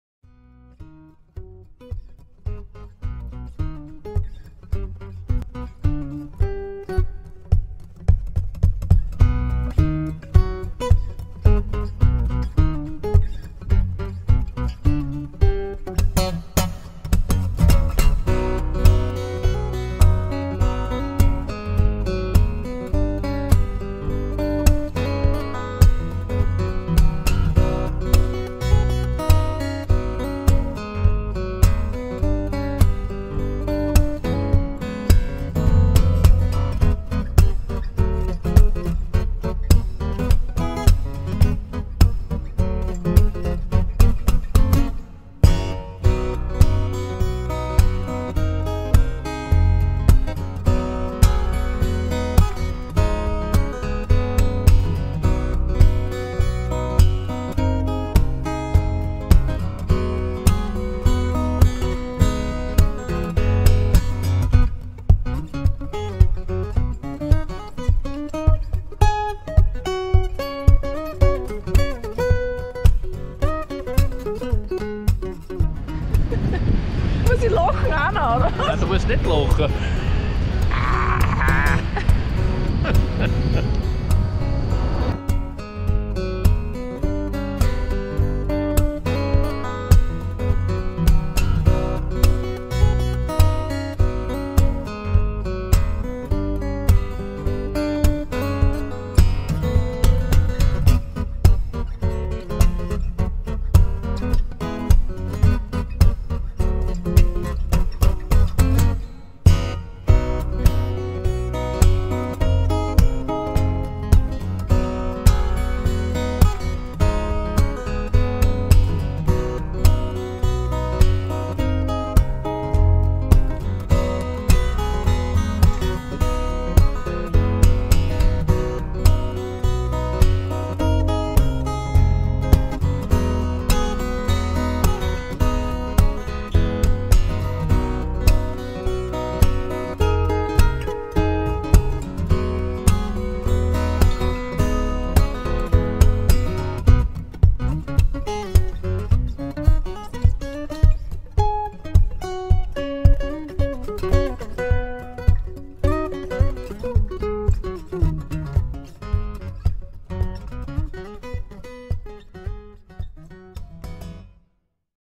UFO Bi- Monte Baldo hike & fly
Schöner und langer hike & fly am Monte Baldo mit Petra!

Monte Baldo: 9 KM, 1700 HM, 2 Std 30 min. Gipfel 1.771 m.

Charakter: Der Aufstieg an der Westseite des Monte Baldo ist steil und schweißtreibend. Ungemein lässige Ausblicke auf den Gardasee und dessen Umgebung entschädigen allemal für diese Tour. Der Startplatz oben ist mega groß und für jeden Piloten geeignet.